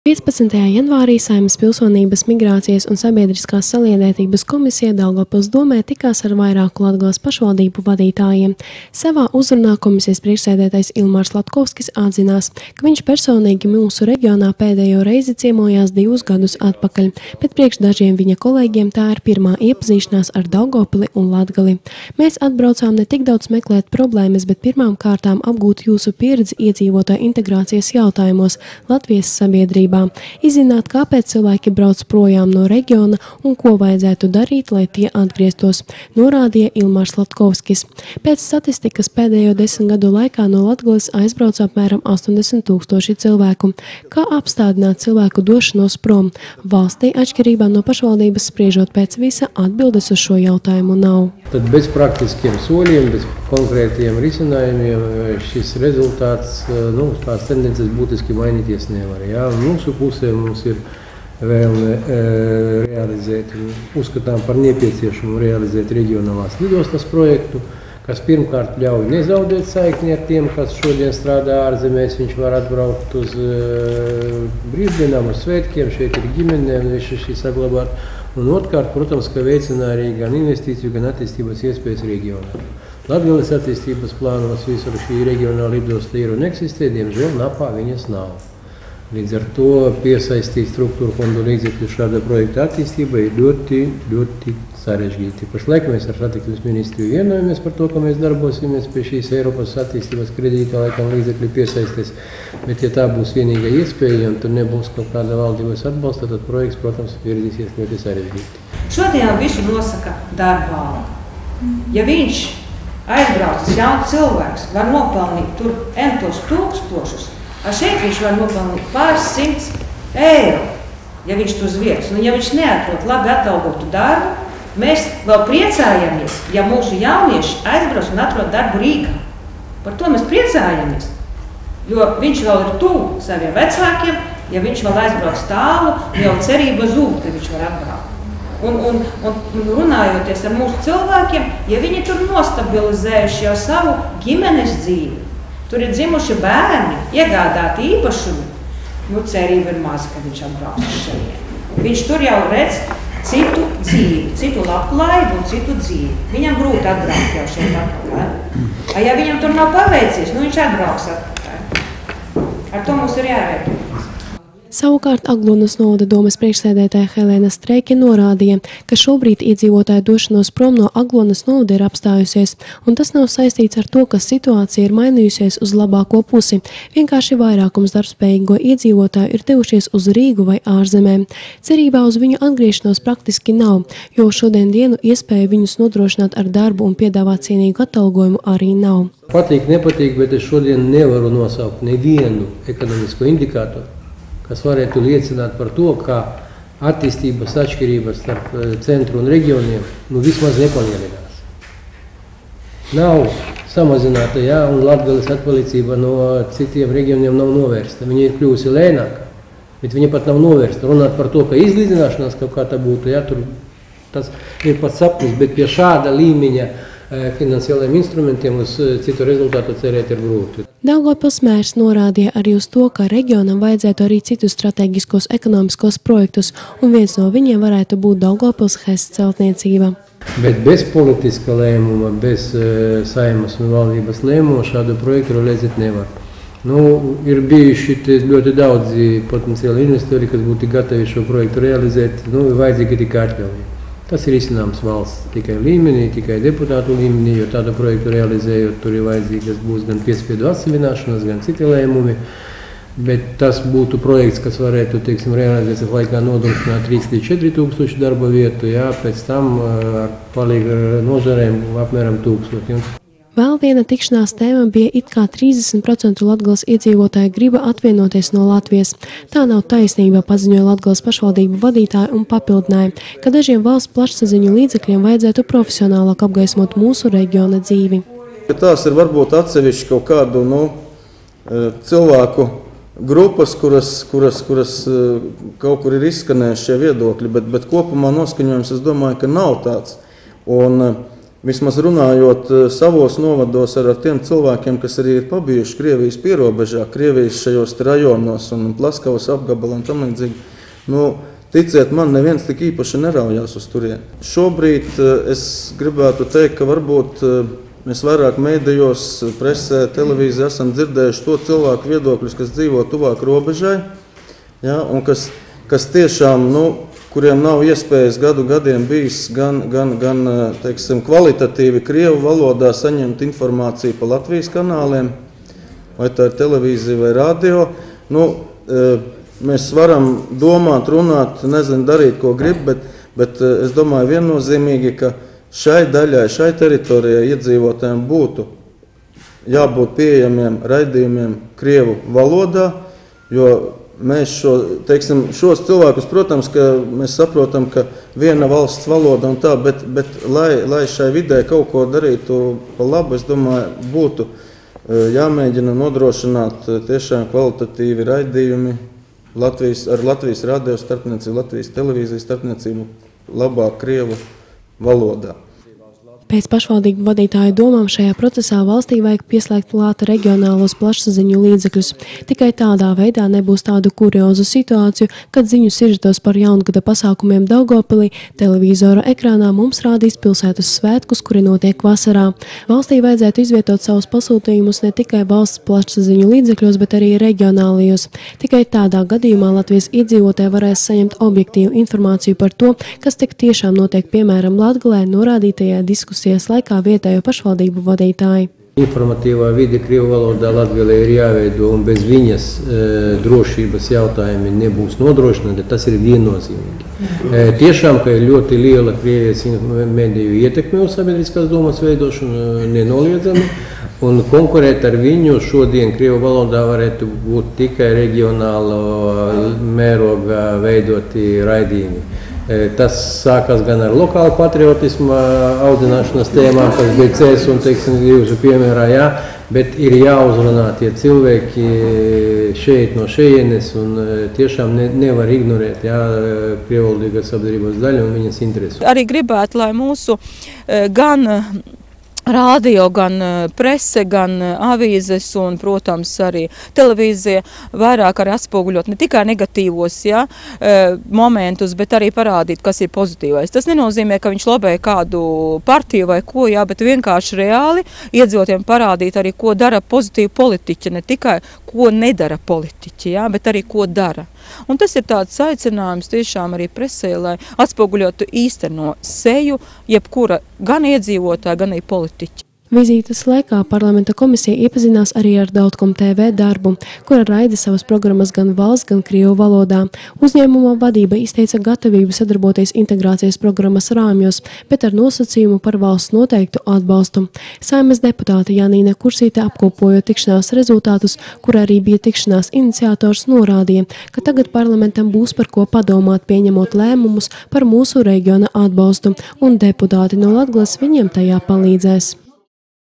Saeimas komisijas izbraukuma sēde Daugavpilī
Saeimas deputāti darba vizītē Daugavpilī. Galvenais vizītes mērķis ir noskaidrot, kāpēc Latgales iedzīvotāji turpina aizbraukt no reģiona un kā efektīvi integrēt sabiedrībā mazākumtautības iedzīvotājus.